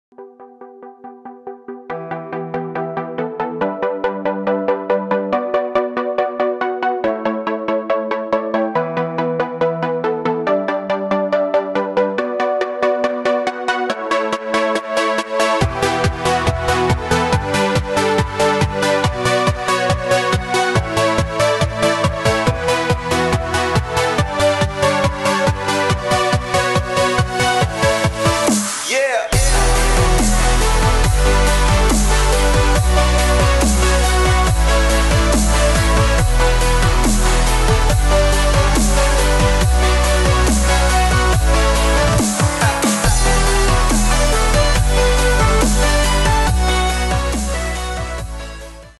LosPolinesios Passes JennaMarbles (16 Minutes Timelapse)
LosPolinesios – Мексиканская звукозаписывающая компания, жанром которой является поп-музыка. Сегодня с 97 места (не считая VEVO-каналов) он перешёл на 96-е

Jenna Nicole Mourey, более известеная по ее псевдониму Jenna Marbles, Американская ютуберша, влогерша, актриса и комик